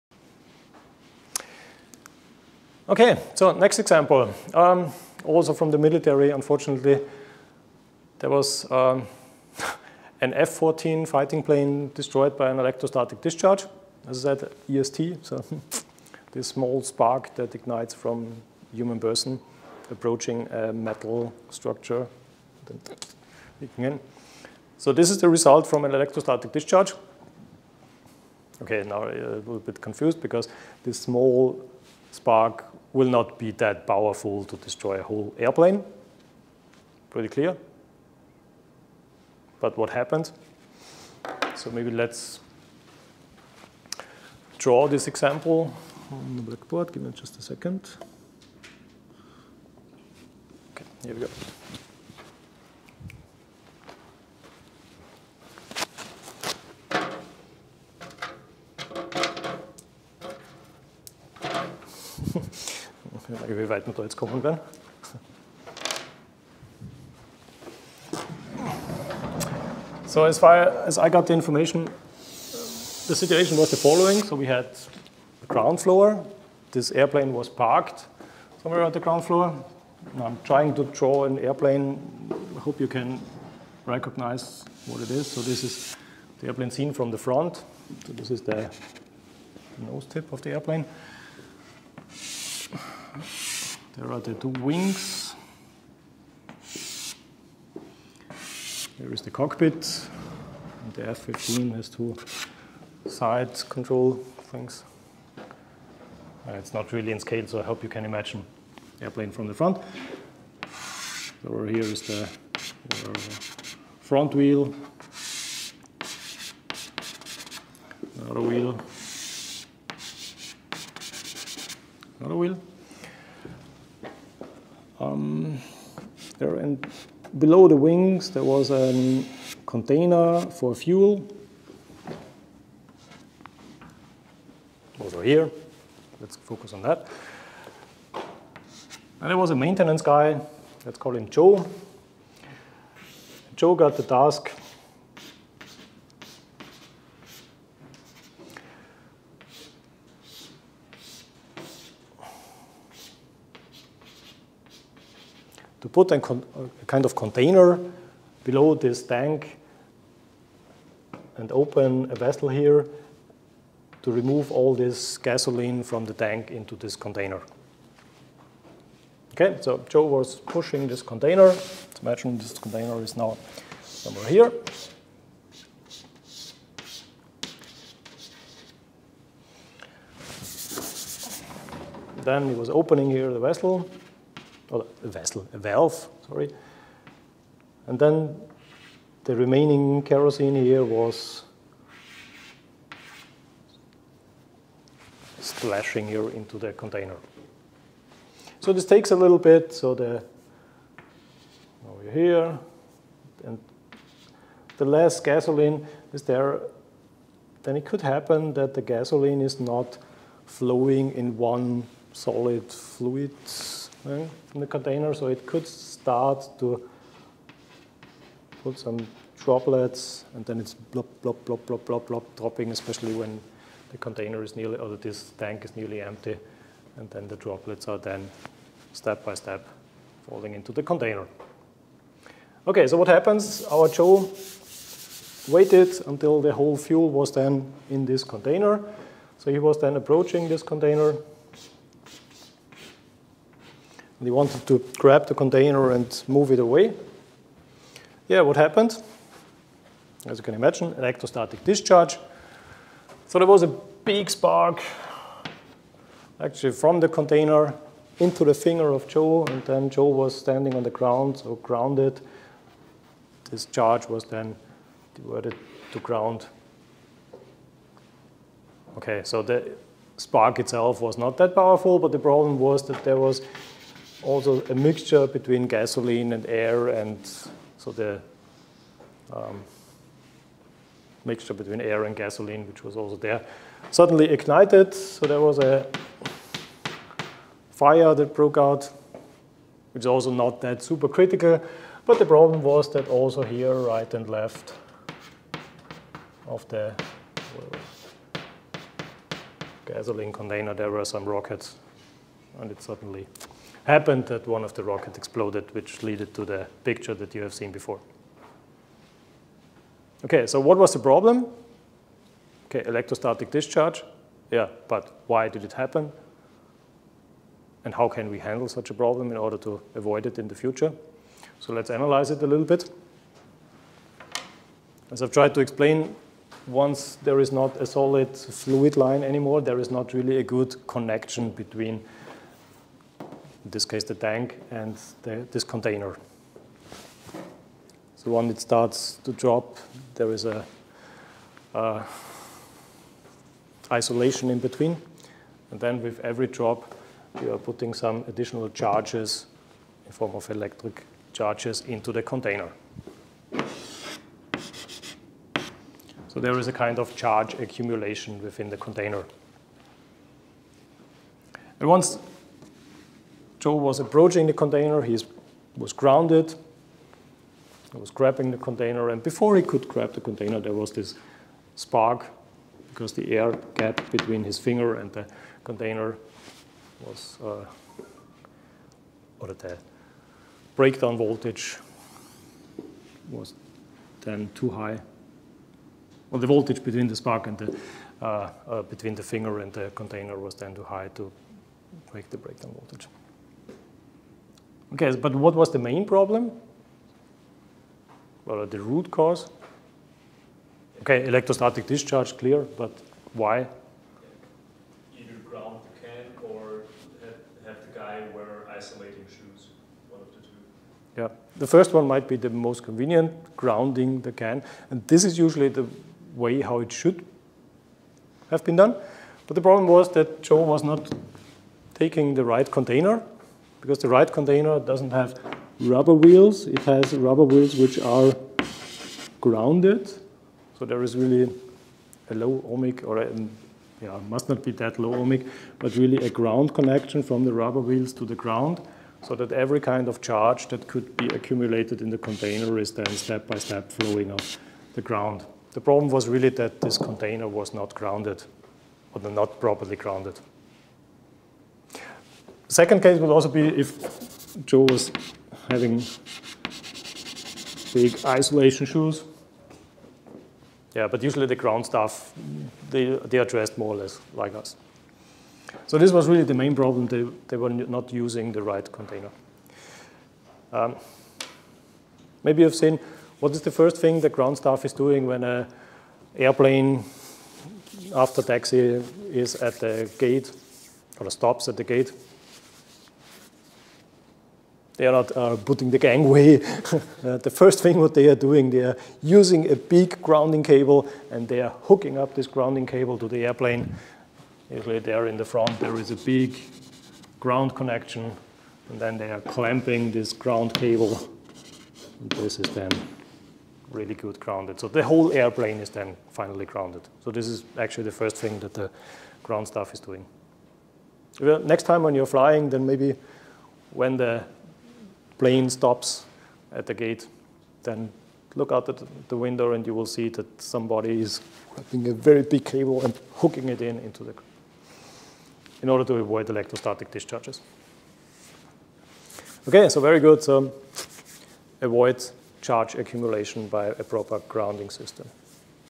EMC Example: Fighter Jet Accident
Bernd Deutschmann
439.210 Electromagnetic Compatibility of ICs

00:00 F14 Example
01:15 Drawing of the Incedent
06:59 Reason of the ESD
07:50 Plane Grounding